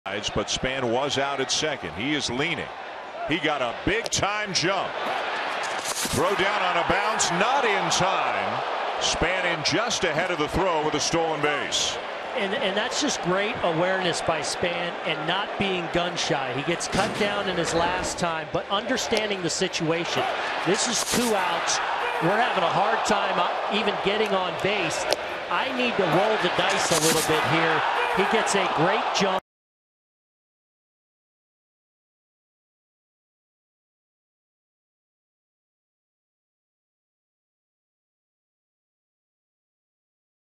Span slides into second with stolen base
Denard Span moves into scoring position as he steals second base on a close play in the 6th inning